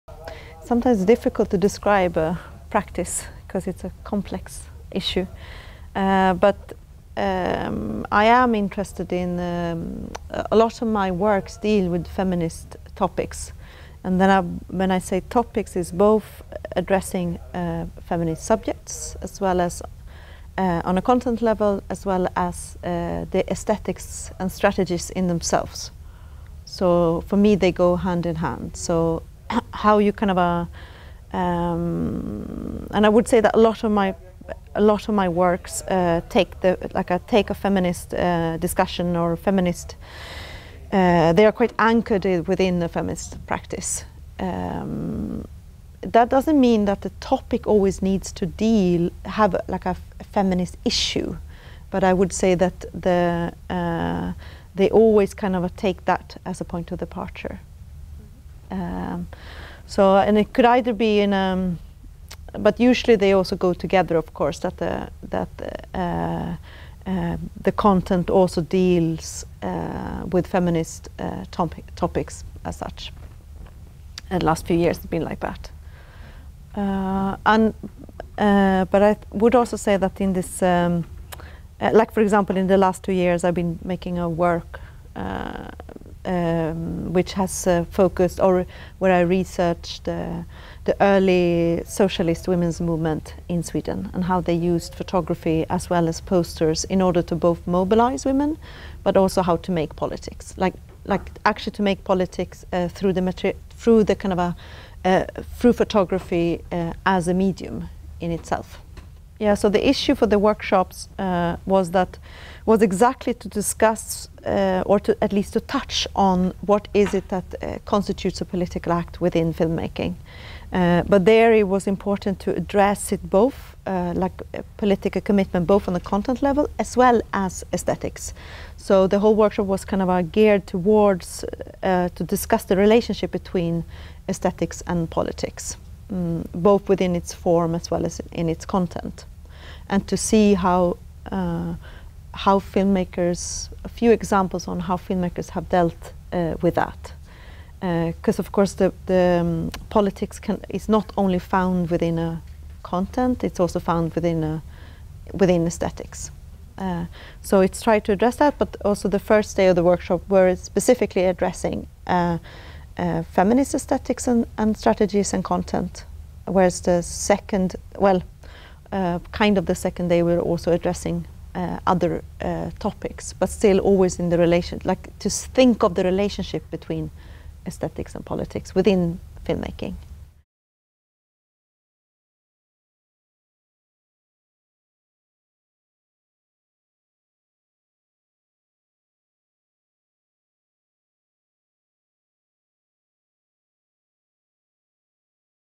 Petra Bauer. LaPublika
Bideo honetan Petra Bauer artistari egindako elkarrizketa ikus daiteke, LaPublika saioen baitan "Las mujeres imágenes en movimiento." saioa gidatu ostean (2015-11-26).

En este vídeo se puede ver la entrevista realizada a Petra Bauer, despues de dirigir una conferencia y talle "Las mujeres imágenes en movimiento." en el marco de las sesiones de LaPublika (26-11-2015).
+info tabakalera.eu/es/las-artes-visuales-como-medios-de-negociacion-politica